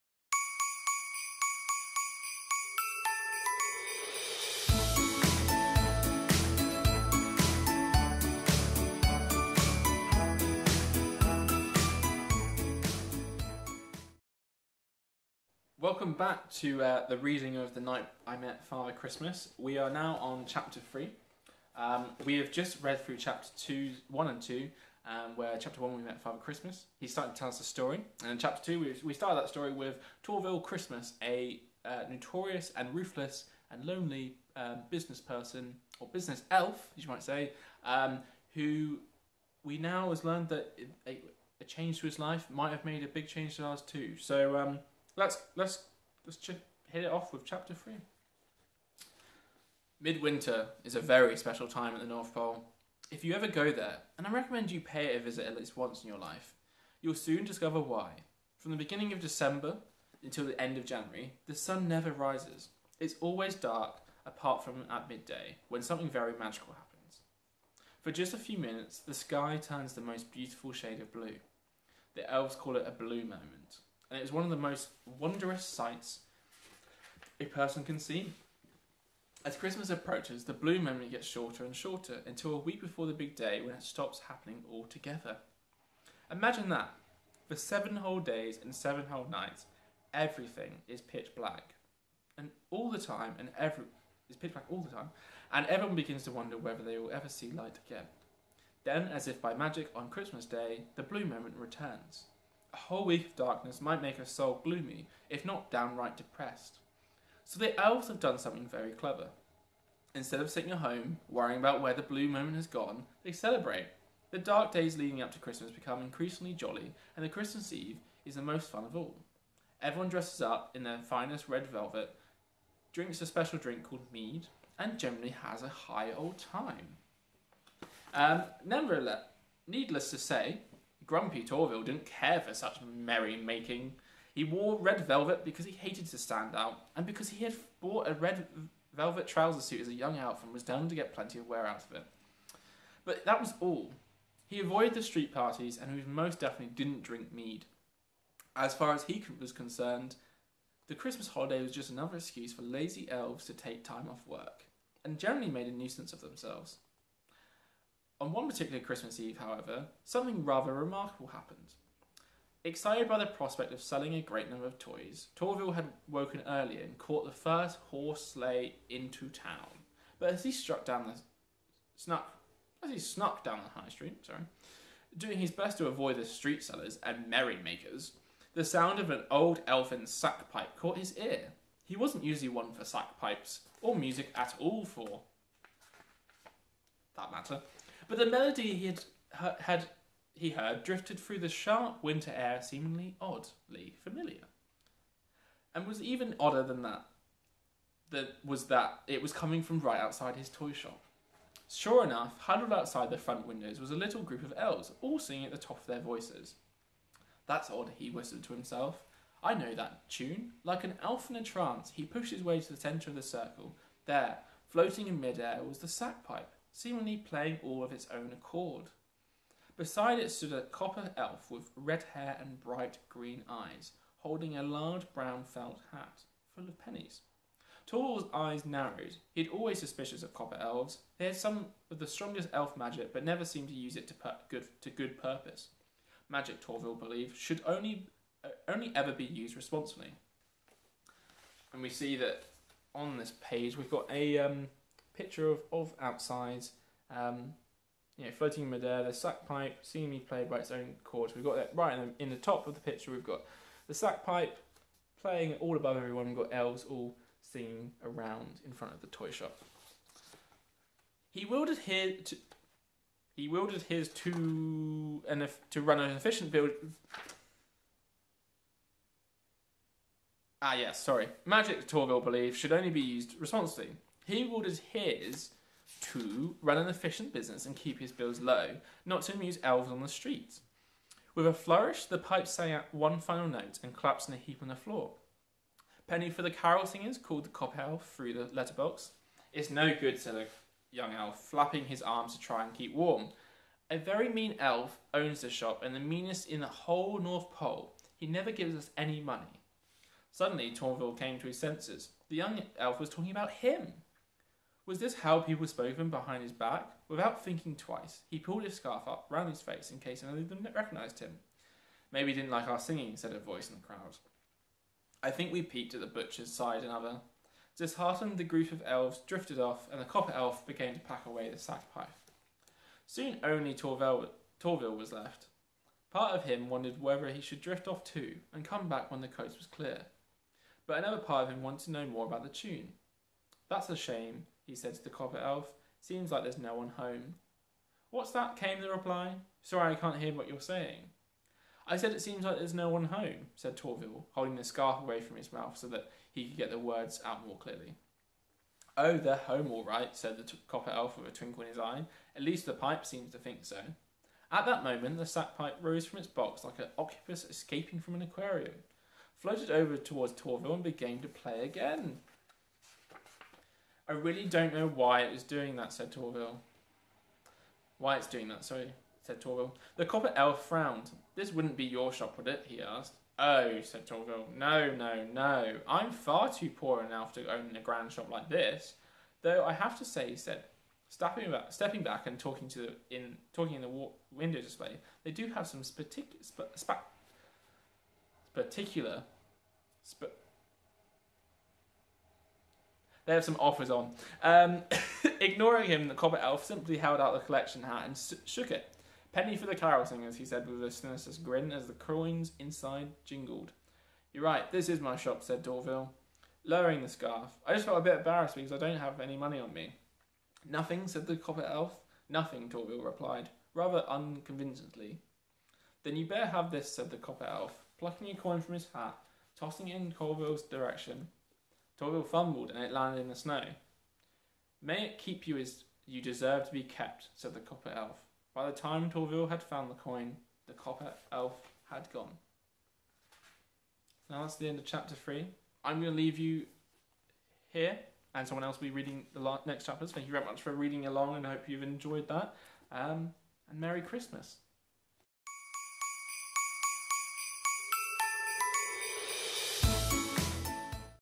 Chapter 3 Complete
The Night I met Father Christmas is written by Ben Miller
Illustrated by Daniela Terrazini
Published by Simon & Schuster
Chapter 3 is read by George